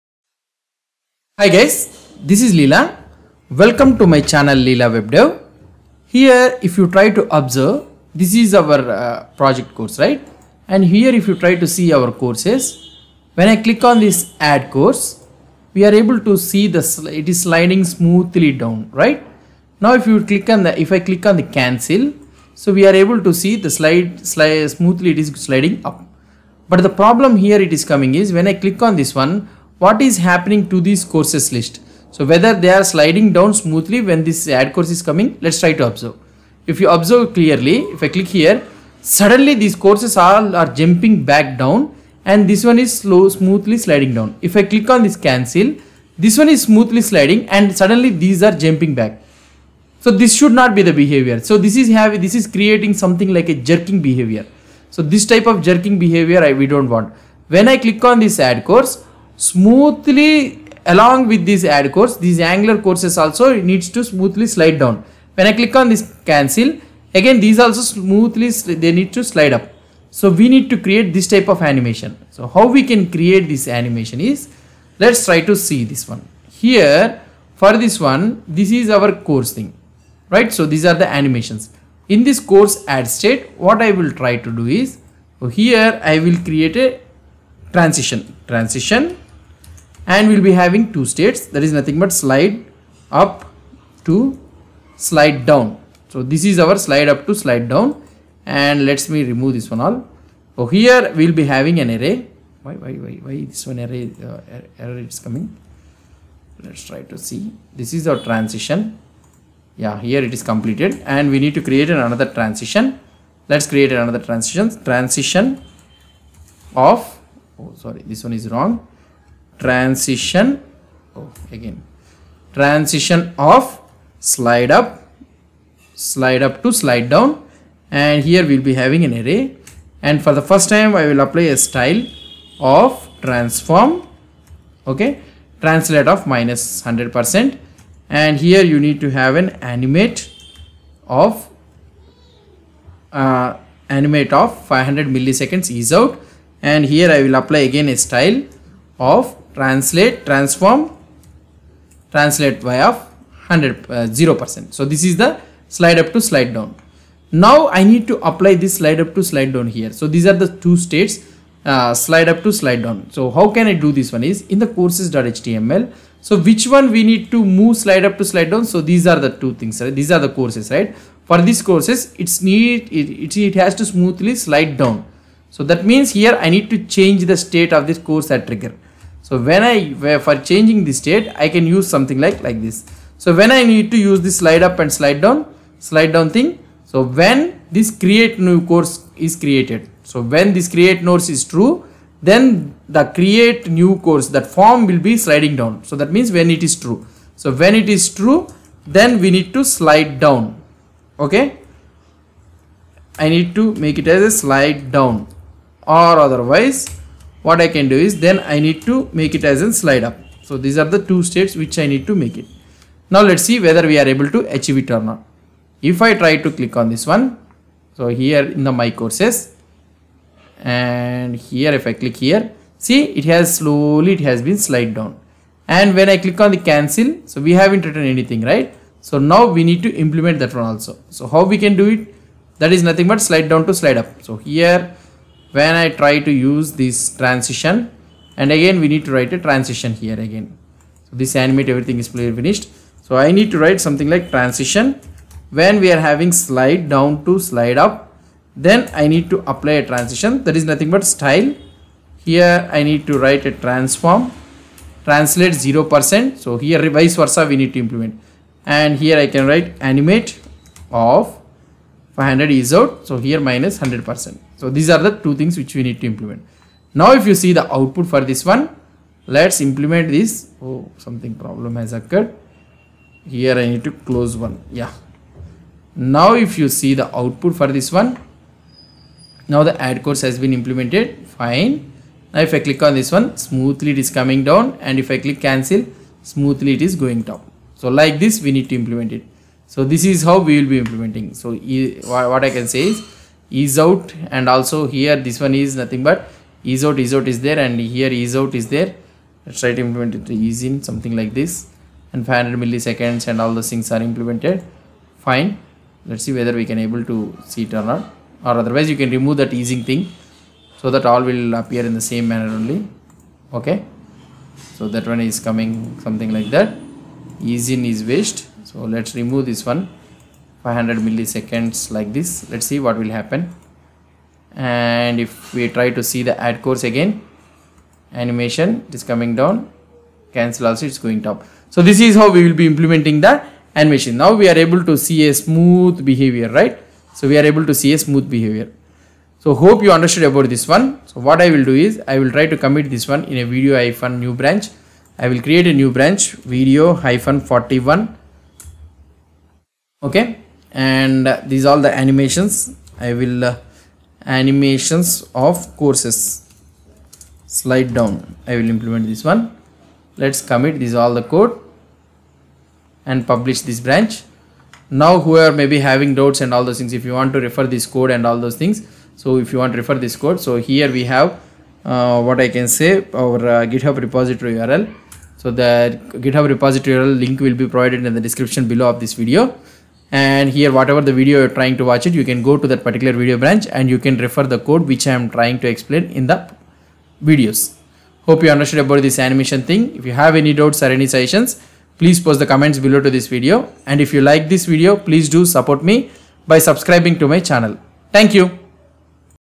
41. Animate the Courses for sliding up and down in Angular Application - Angular16
In this video we will see how to Animate the Courses for sliding up and down in Angular Application - Angular16